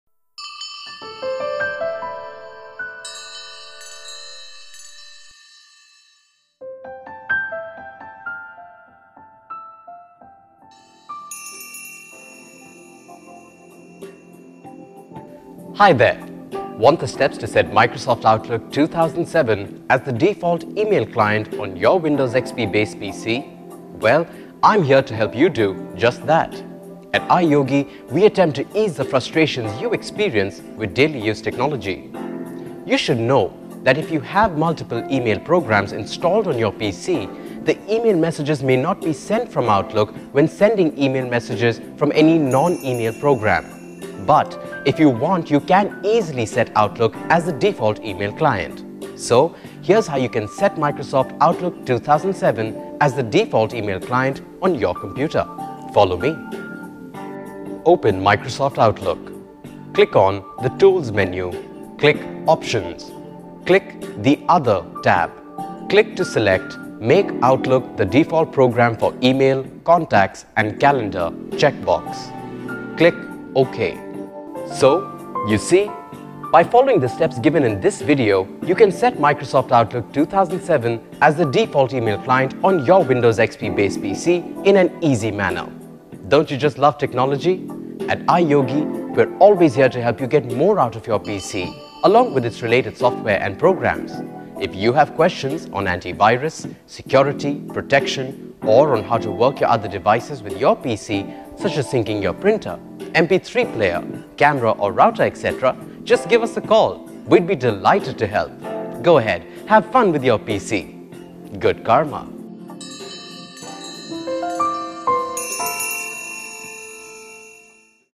Set Microsoft® Outlook 2007 as Default Email Client on Windows® XP
If you are looking for the steps to set Microsoft® Outlook 2007 as the default e-mail client on a Windows® XP based PC, watch this video.

If you have multiple e-mail programs installed on your PC, the e-mail messages may not be sent from Outlook when sending e-mail messages from any non-e-mail program.